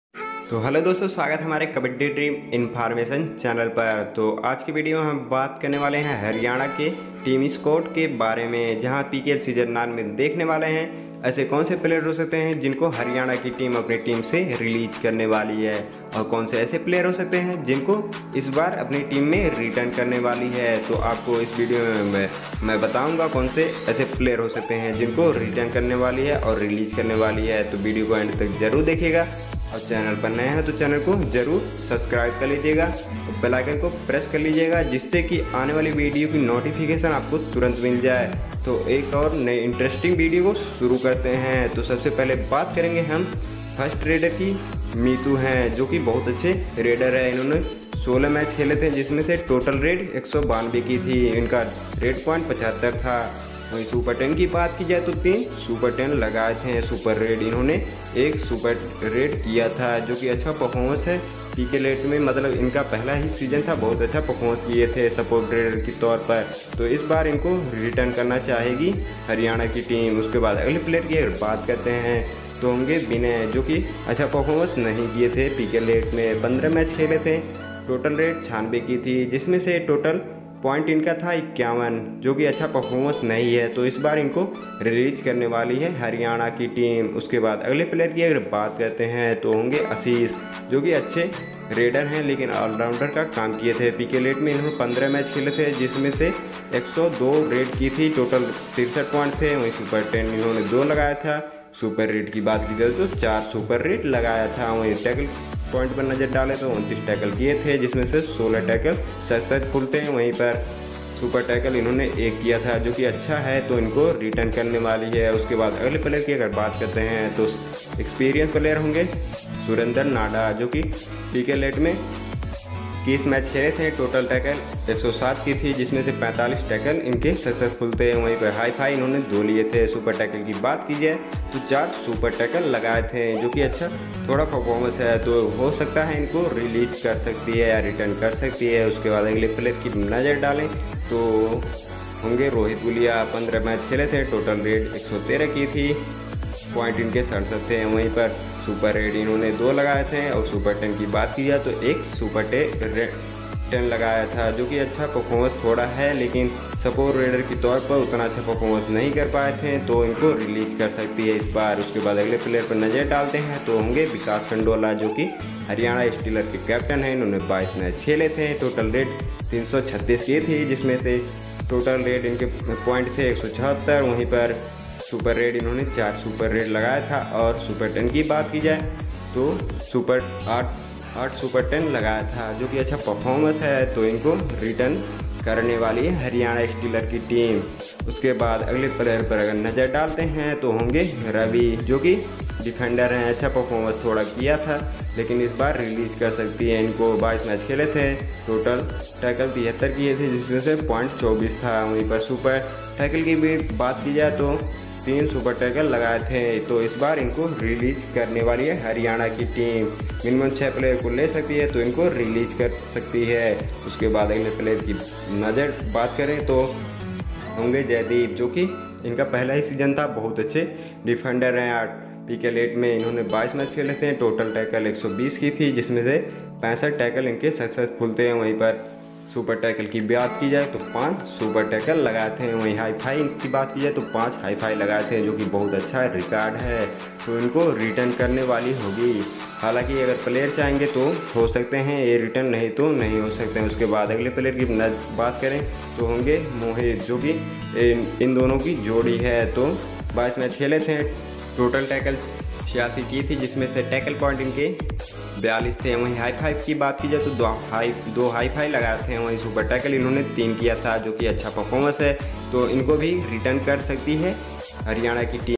Haryana steelers Team season9 2022 इन खिलाड़ियों को रिटर्न करेंगी हरियाणा की टीम और इनको बाहर करेगी
Haryana steelers Team squad season 9 2022

बेंगलुरु बुल्स की टीम इन खिलाड़ियों को रिटर्न नहीं करने वाली है